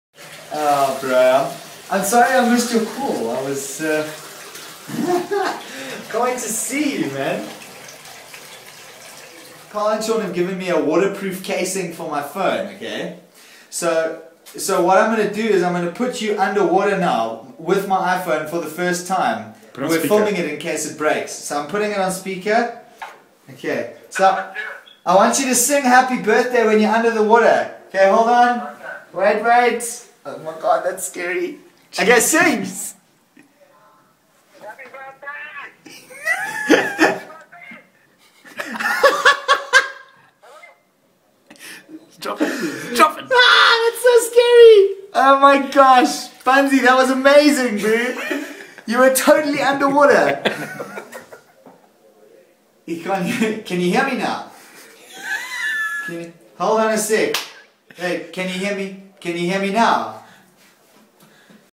Underwater iPhone Birthday Message - TPU Waterproof Case Test
Bunz wishing Mavis happy birthday, in the sink.